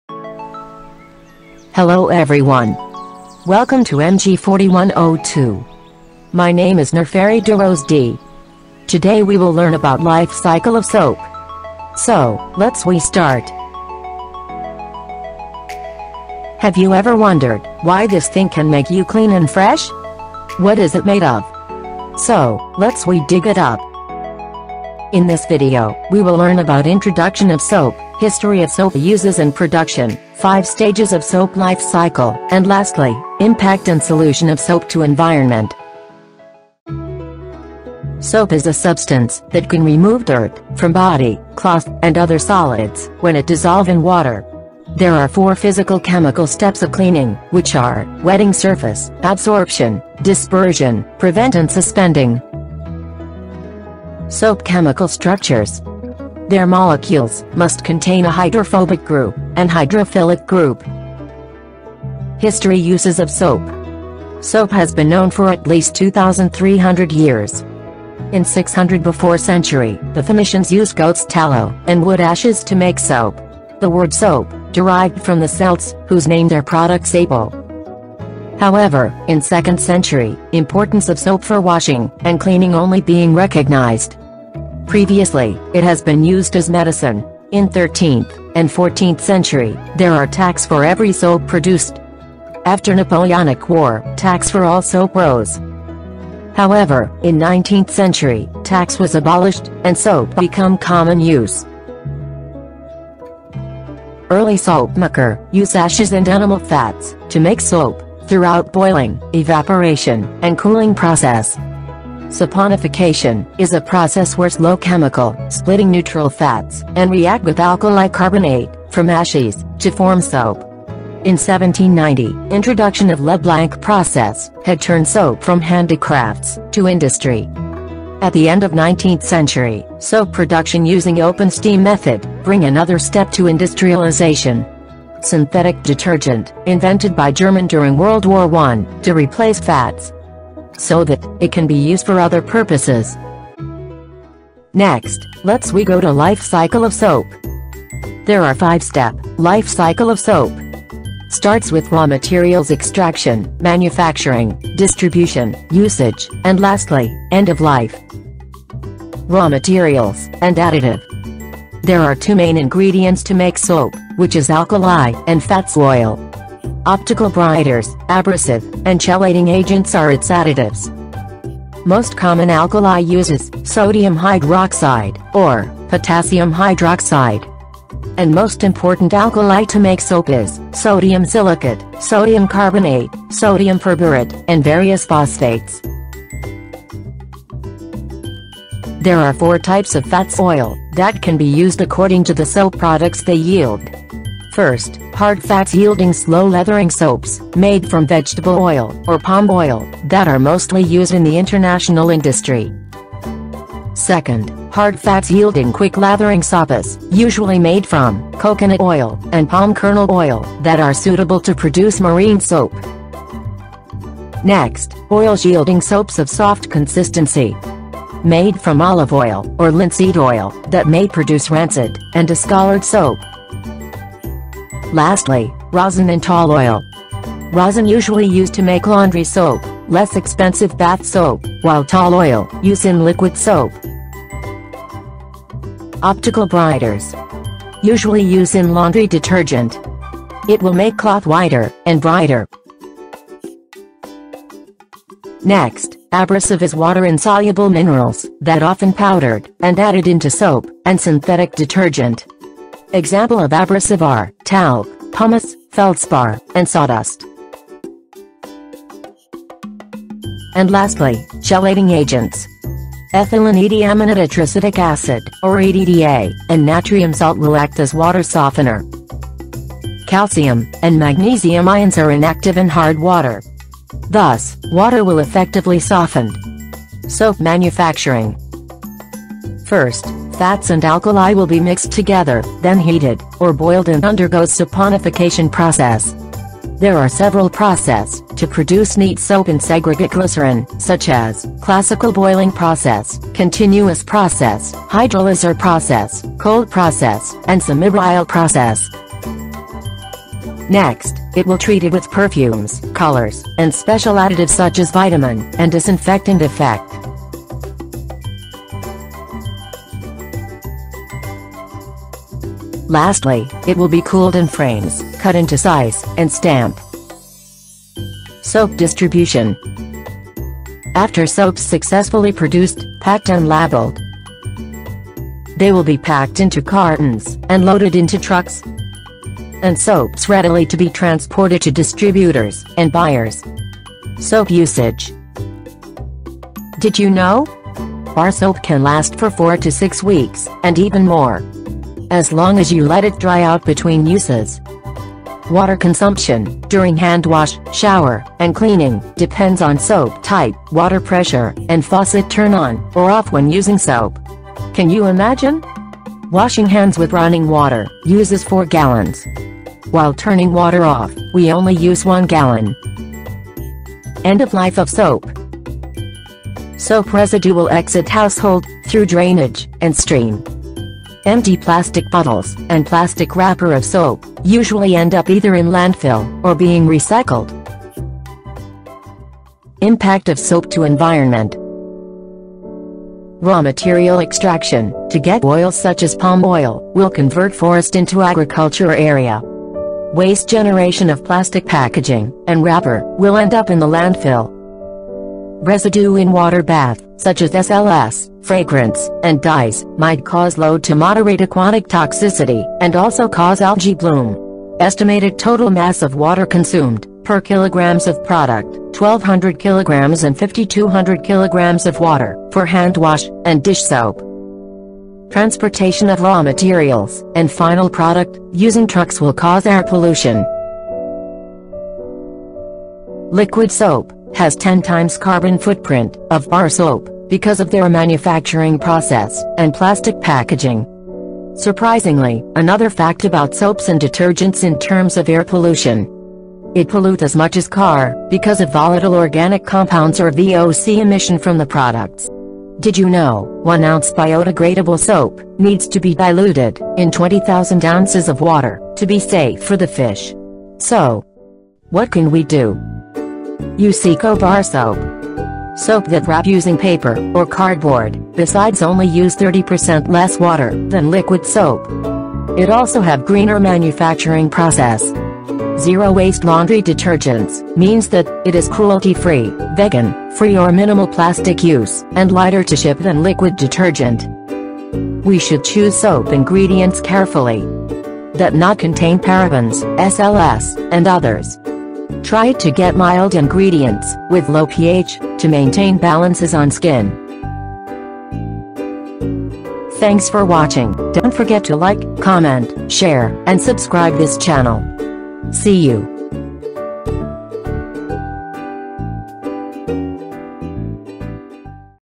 Life cycle assessment (LCA) of soap
What is elegance? Yeah, soap and water! Soap is a symbol of hygienic tools in our daily lives. It can remove dirt and freshen us up with a tempting smell. However, did you know that soap also can give adverse impact to our environment? Here, the video about LCA of soap for you to learn more details about soap.

Introduction- 0:36
History- 1:01
Life cycle- 2:25
Impact- 6:20
Solution- 7:31

Thank you for your time and feedback. Hope to hear from you soon.